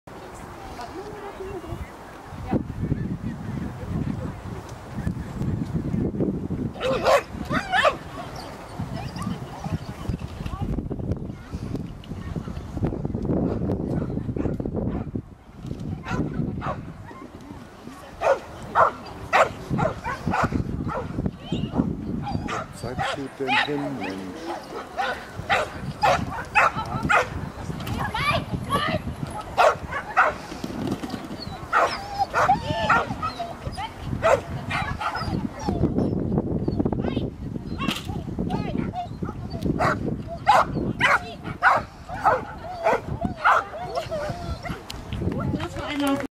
11.08.2013 GHSV Blumberg
Blumberger Ostercup im August, A2-Lauf, V5 (eine Stange), Platz 2